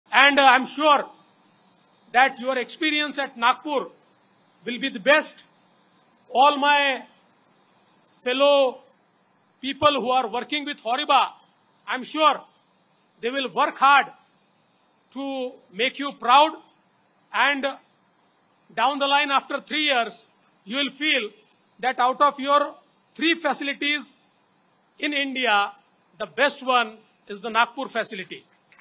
HORIBA will achieve new heights of success! | Nagpur | DCM Devendra Fadnavis
(Inauguration ceremony of HORIBA India Nagpur Facility | Nagpur | 6-7-2024)